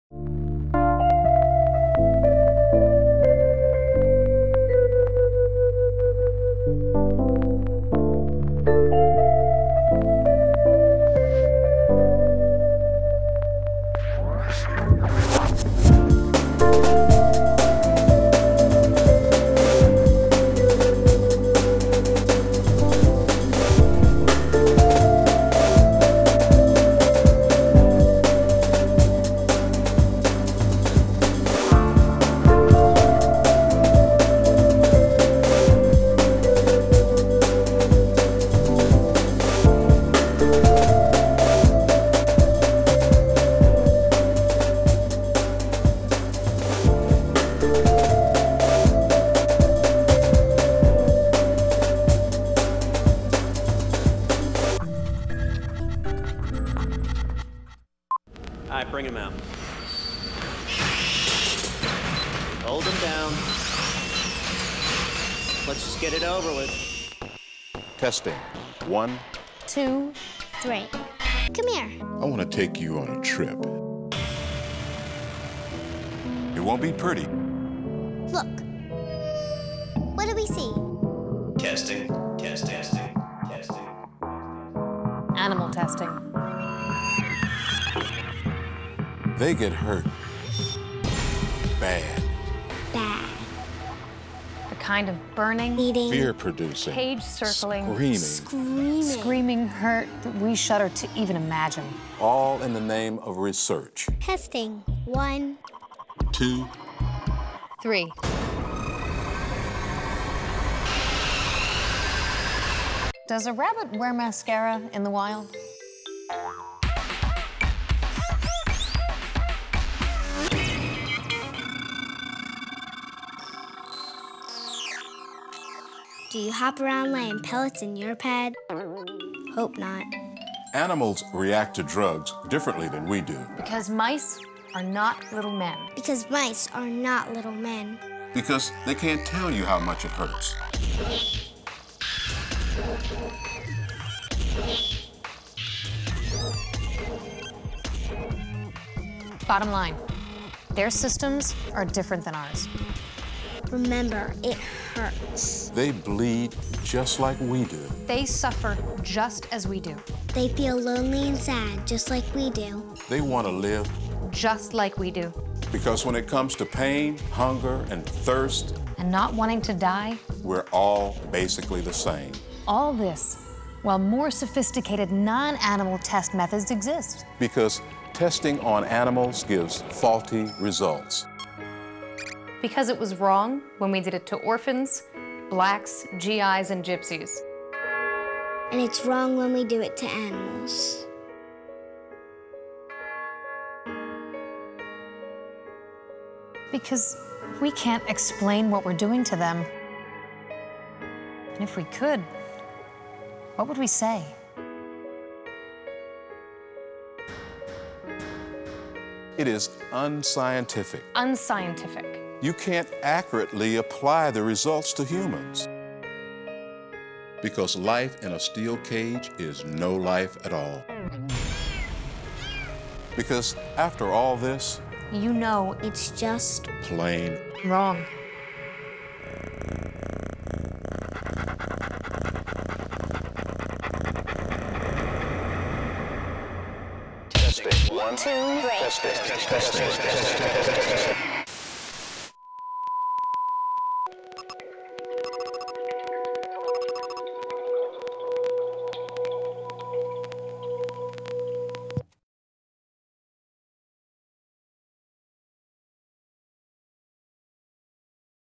Nestea CruelTEA
Nestea torments animals in deadly tea tests. We need your help to stop the abuse!